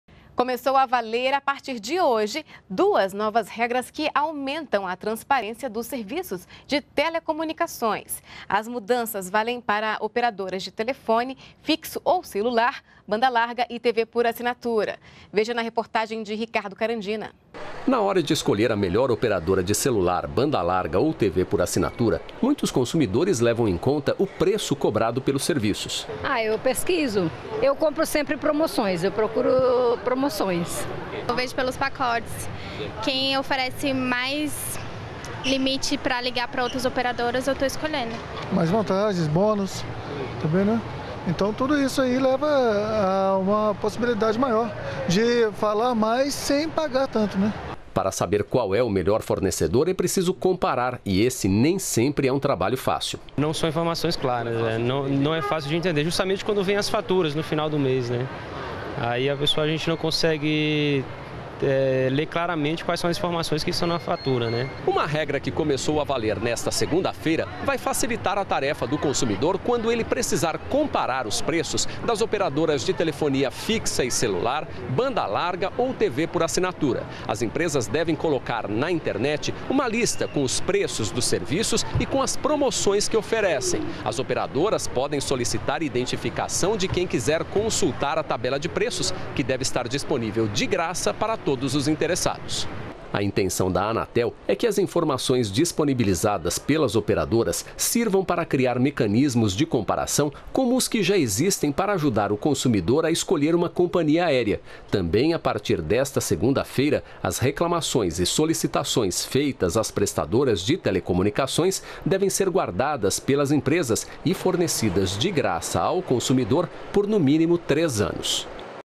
Entram em vigor novas regras para serviços de telecomunicações
NBR NOTÍCIAS - 10.11.14: A partir desta segunda-feira (10), entram em vigor novas regras que devem aumentar a transparência nas relações de consumo dos serviços de telecomunicações. As empresas agora devem disponibilizar na internet uma lista com os preços dos serviços e promoções que oferecem, facilitando a comparação de preços pelos consumidores. Além disso, todas as reclamações, pedidos e solicitações efetuados às prestadoras devem ser guardadas pelas empresas e fornecidas ao consumidor, sem ônus, por um período mínimo de três anos. As mudanças valem para operadoras de telefone fixo ou celular, banda larga e TV por assinatura.